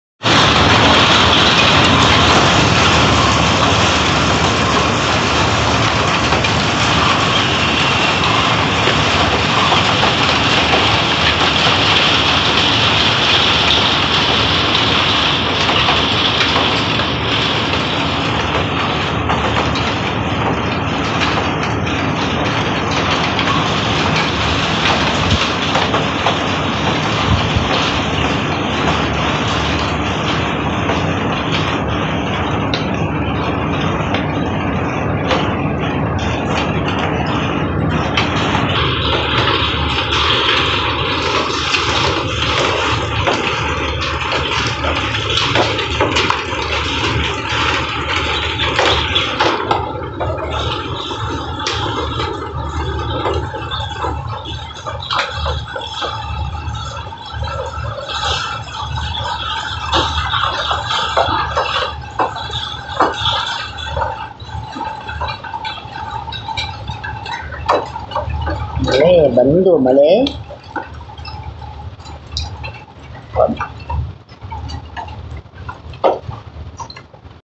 Rain Says "I just landed !!!" Because it's new year !! Special Gift for Ugadi, Vishu and Puthandu 🙏
Summer Rain hits Bangalore  🌧️🌧️🌧️🌧️
Summer Rain 2021